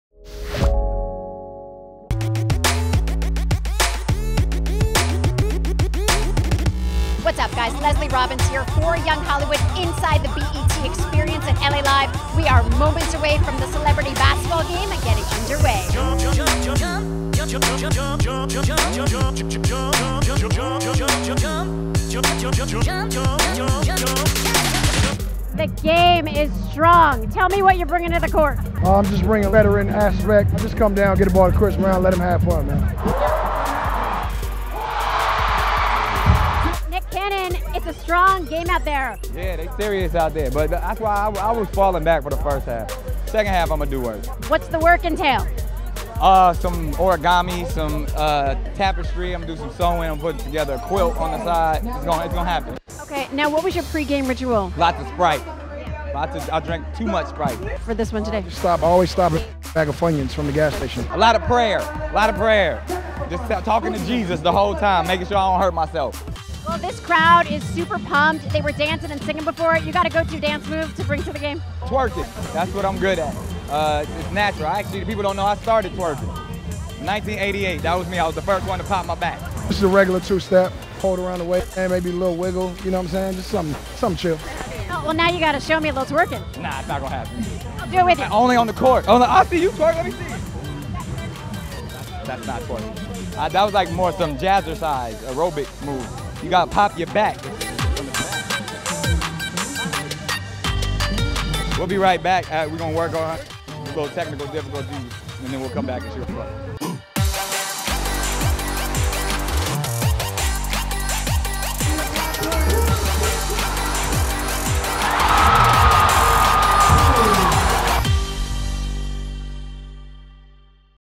Pre-BET Celeb Game Rituals With Nick Cannon & The Game
YH hits the court at the BET Experience at L.A. Live where celebs like Nick Cannon and The Game are getting ready to take part in the celebrity basketball game! We catch up with the comedian and the rapper to find out their pre-game rituals and on-court dance moves!

Apps

2&keywords=young+hollywood

Social

Channels